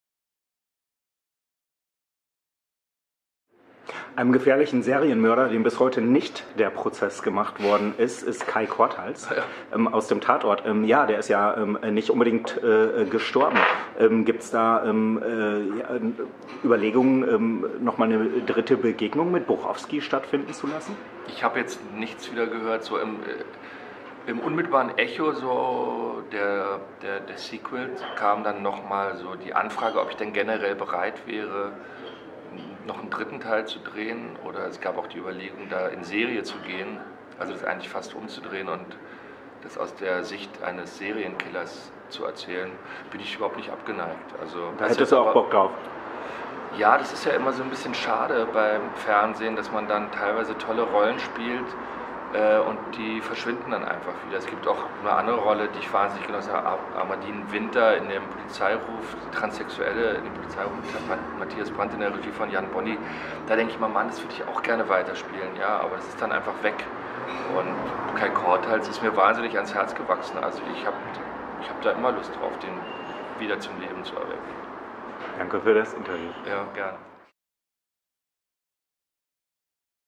Lars Eidinger alias Kai Korthals im Interview
Lars Eidinger alias Kai Korthals war der gruseligste "Tatort"-Mörder aller Zeiten. Wird es eine dritte Begegnung mit Borowski geben oder geht Kai Korthals sogar in Serie?